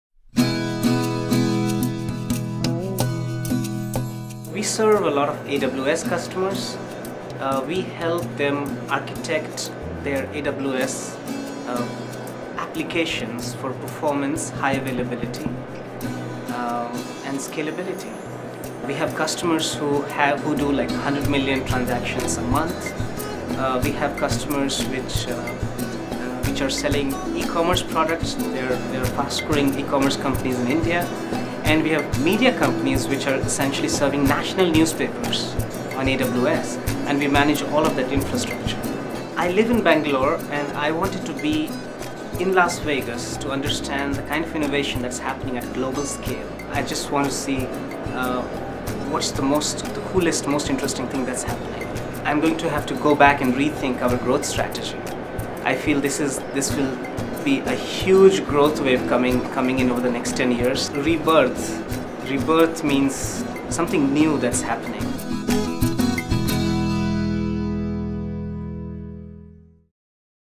Kuliza Technologies on AWS - Customer Success Profile
Learn how Kuliza Technologies is able to grow by taking advantage of AWS and the benefits of cloud computing.